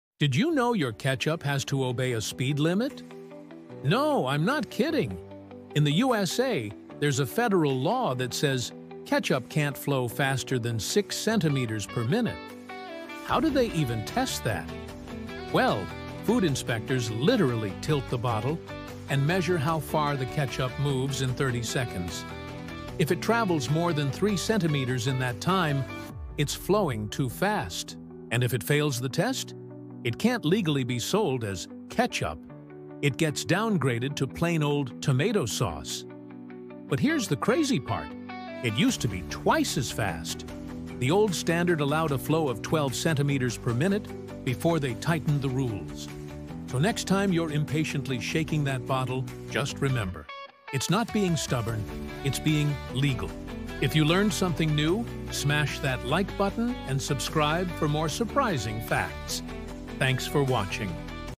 The Secret Ketchup Speed Law 🍅🇺🇸
In the U.S., ketchup has a legal flow rate! By law, it can’t move faster than 6 cm per minute — or it’s not officially ketchup. This bizarre rule was created to ensure that ketchup stays thick and rich, not runny like tomato sauce.